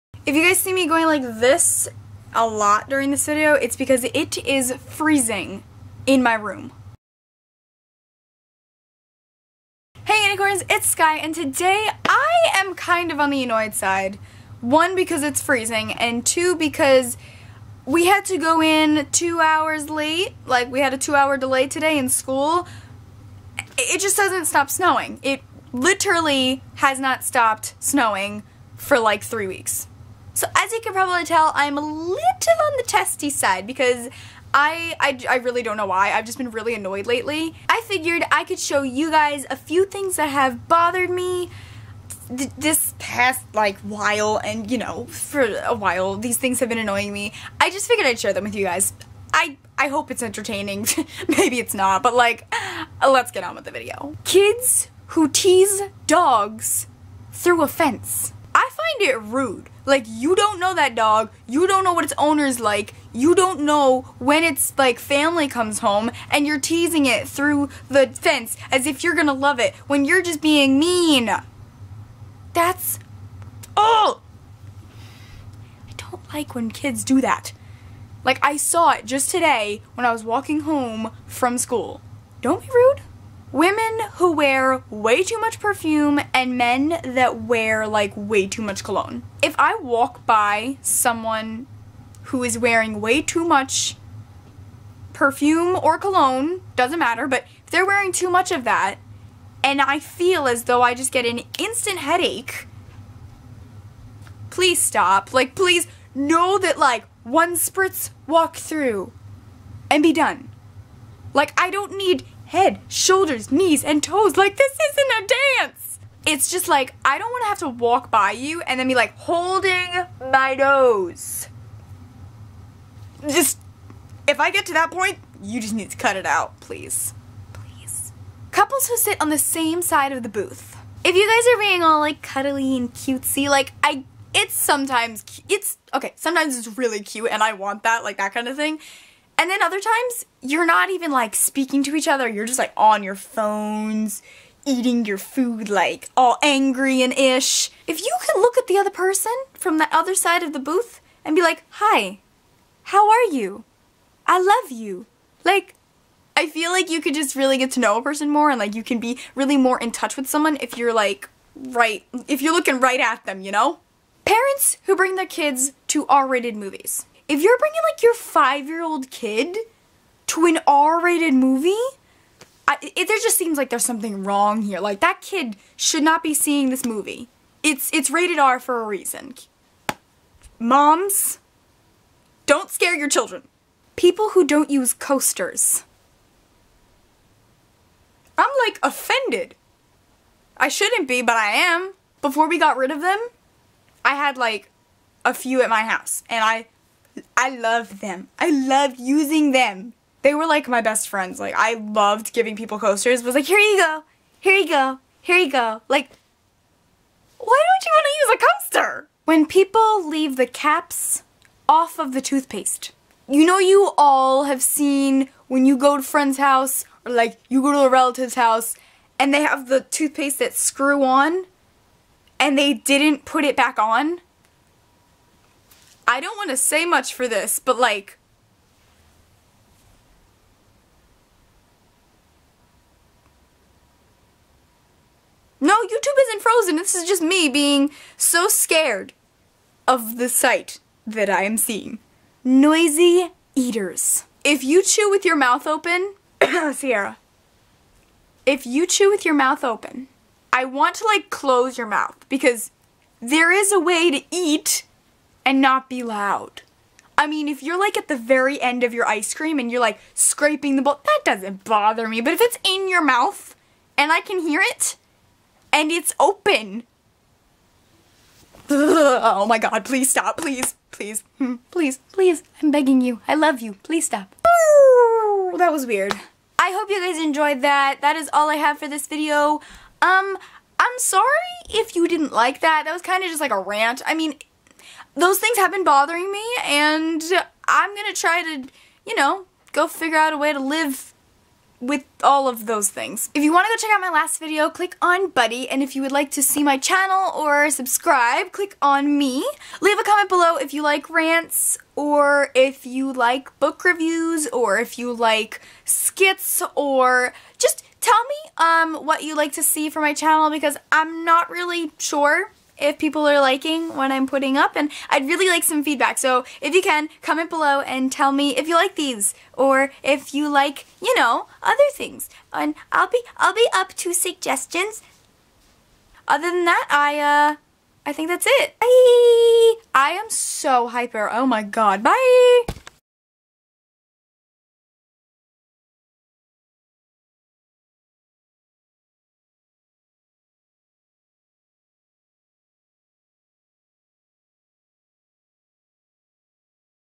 I Hate When People Do This || Skye M
hope you guys enjoyed this little bit of a rant // love you guys so muchh :)

leave a comment of some things you would like to see for my channel!!